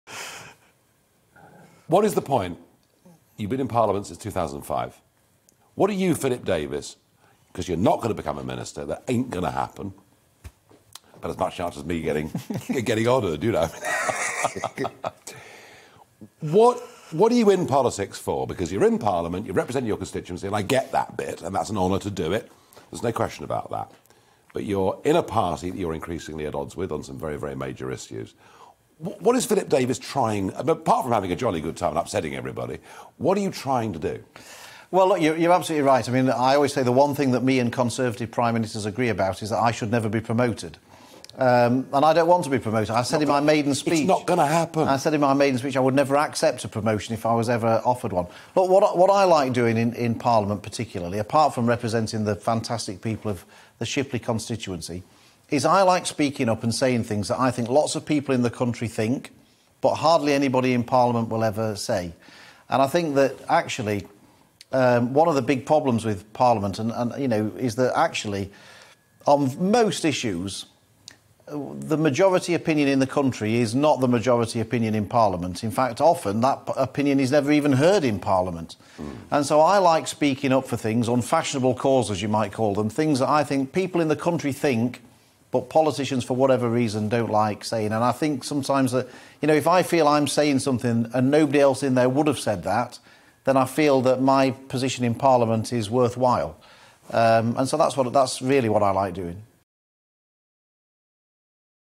Philip Davies: I like speaking up for things people in the country think but politicians don't like
‘I always say the one thing that me and Conservative Prime Ministers agree about is that I should never be promoted’

Conservative MP Philip Davies on what he aims to achieve in Parliament.